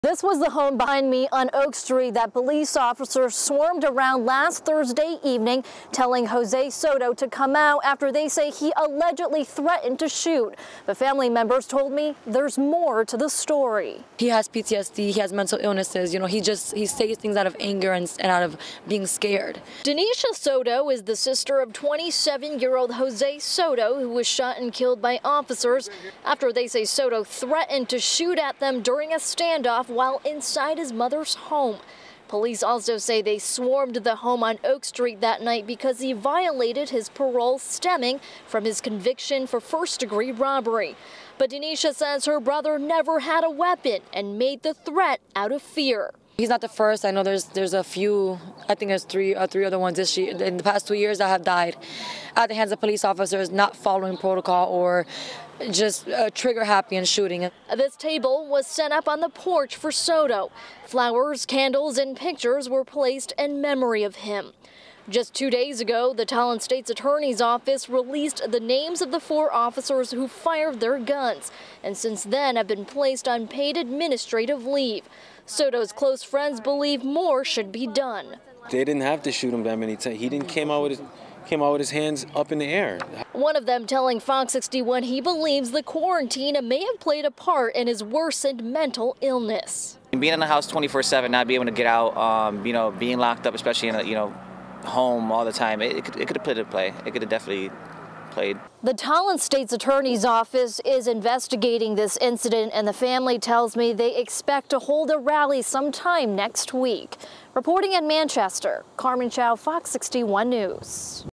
Friends and family hold a memorial for man shot and killed in manchester officer-involved shooting
Police said Jose Soto, 27 violated his parole for conviction of first-degree robbery.